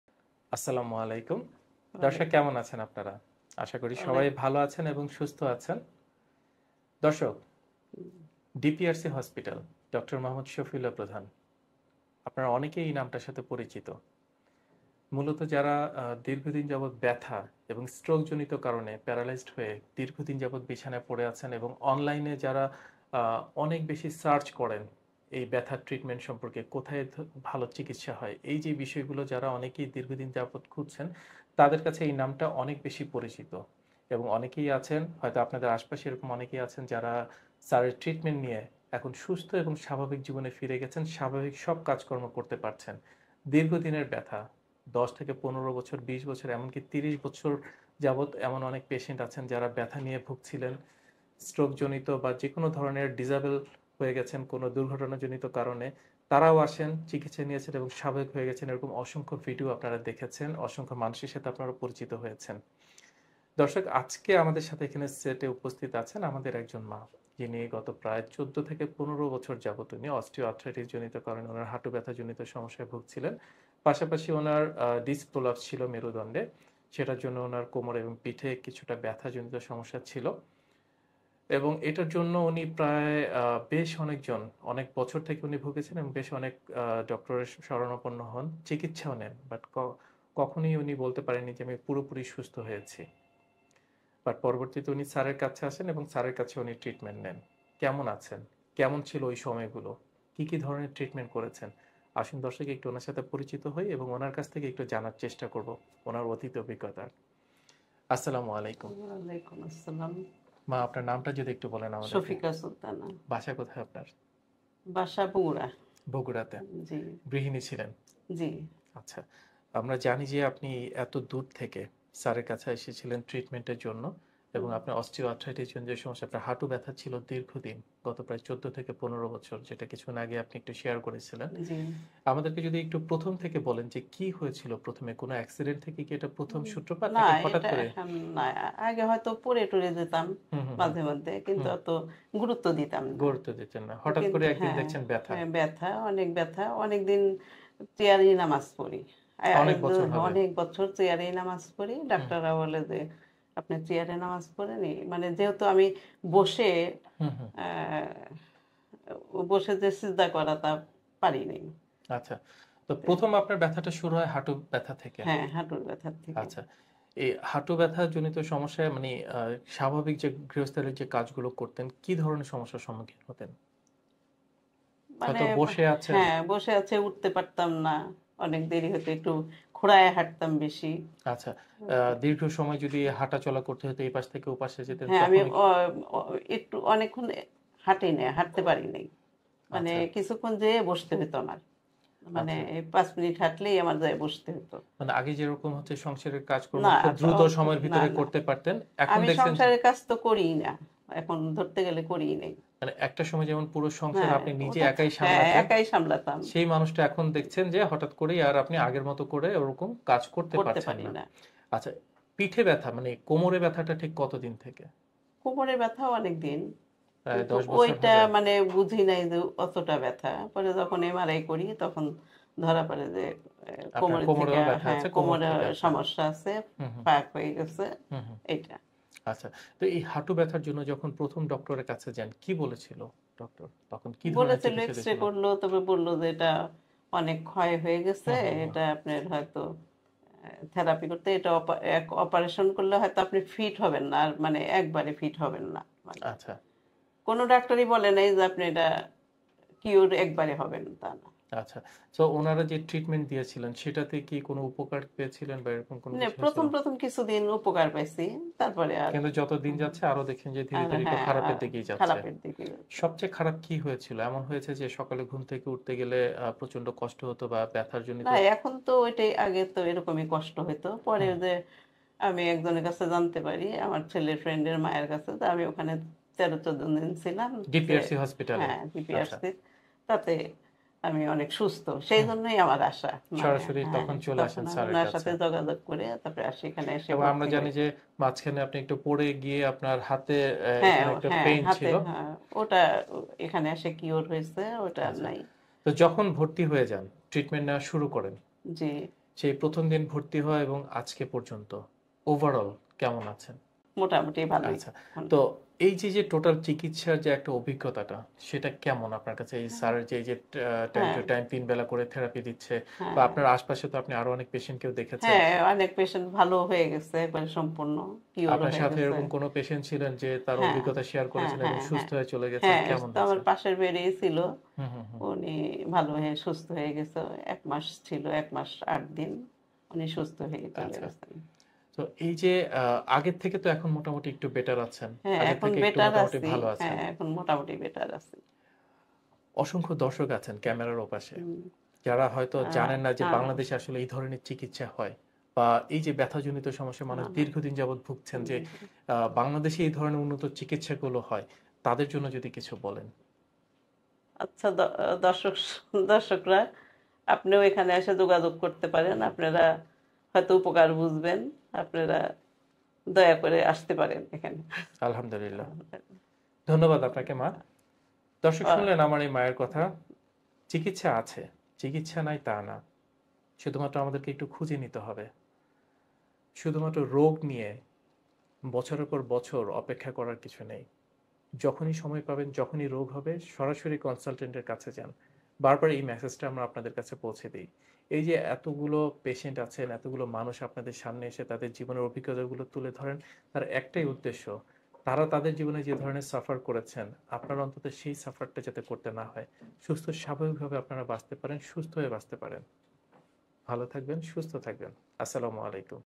দীর্ঘ দিনের হাটু ব্যথা থেকে মুক্তি পেলাম আলহামদুলিল্লাহ | Dr.Md.Shafiullah Prodhan | DPRC Hospital
✺আমাদের  এর ঠিকানাঃ-
DPRC
12/1 Ring Road, shyamoli Dhaka-1207, Bangladesh.
(ডিপিআরসি , 12/1  রিং রোড শ্যামলি ঢাকা-১২০৭ বাংলাদেশ)